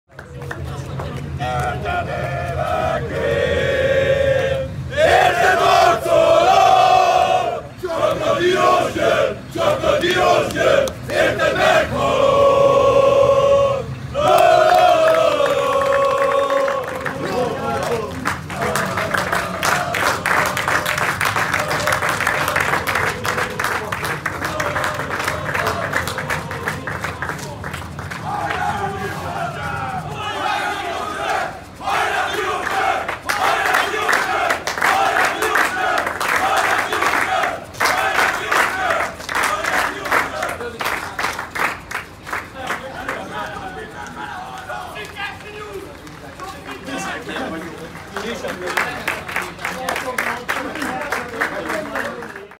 Elindult a DVTK Székesfehérvárra | DVTK TV | 2020. március 13.
Ilyen kíséret mellett indult útjára a DVTK labdarúgó csapata Székesfehérvárra, ahol szombaton játszik bajnoki mérkőzést a DVTK. 🔴🔥⚪